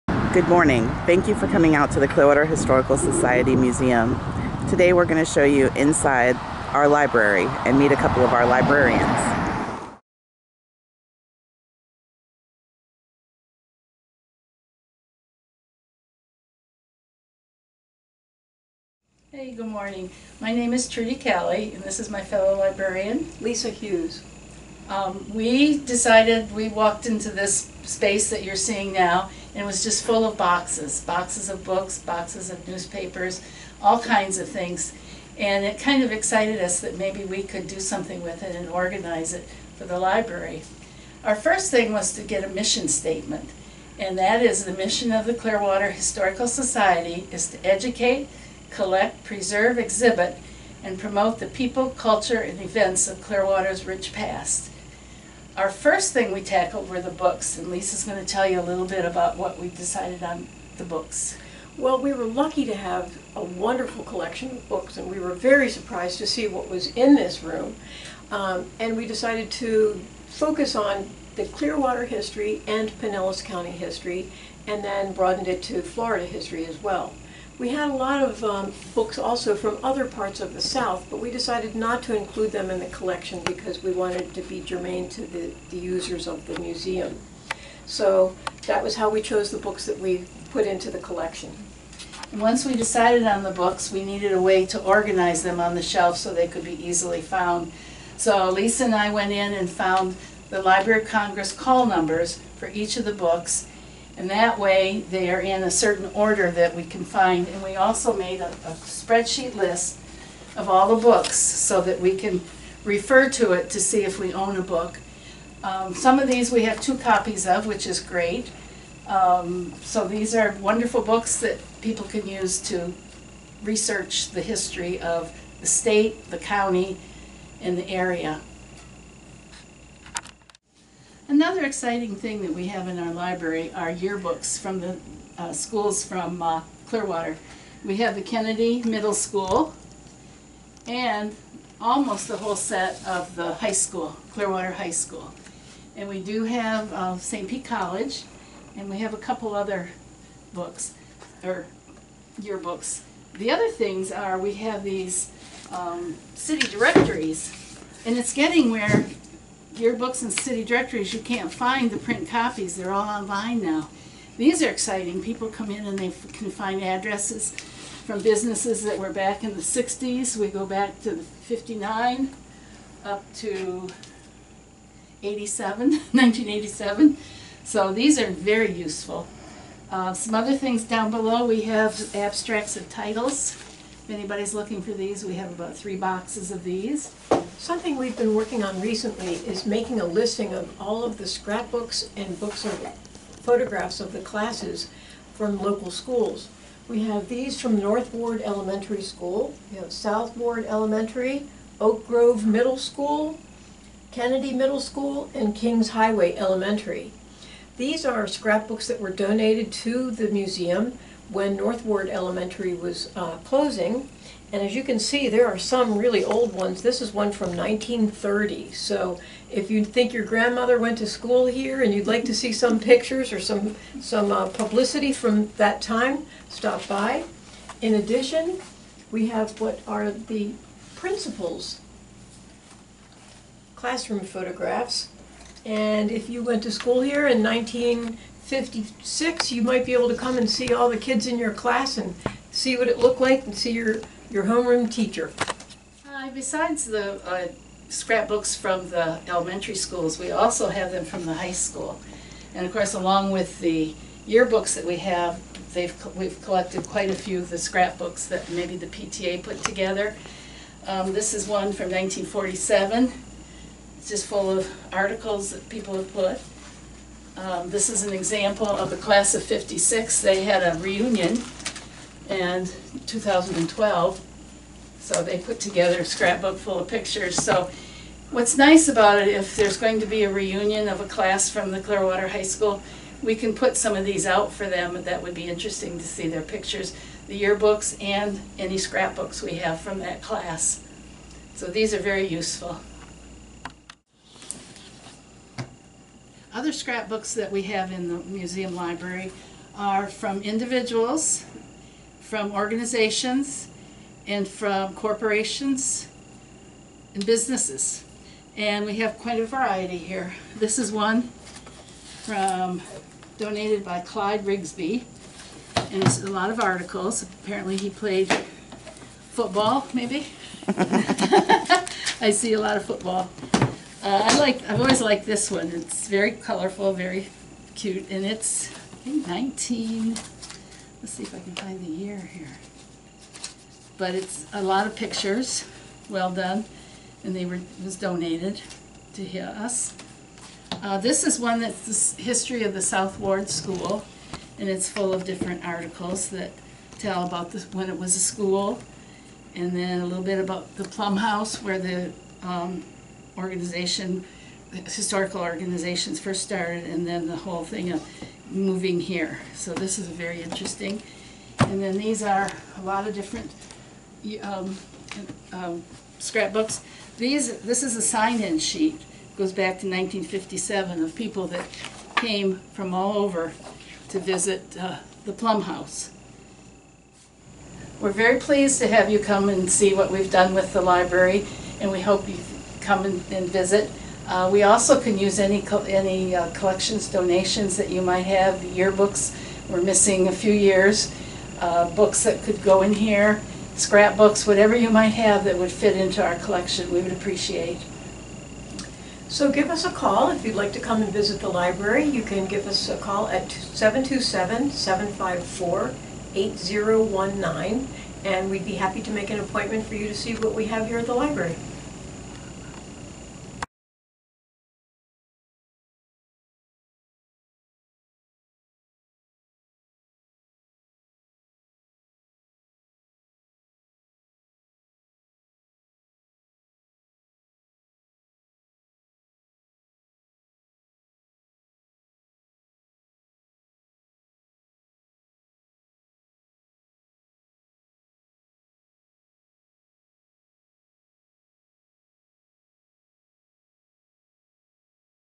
Clearwater Historical Society Library Tour Video
This is a visual tour of the Clearwater Historical Society's library.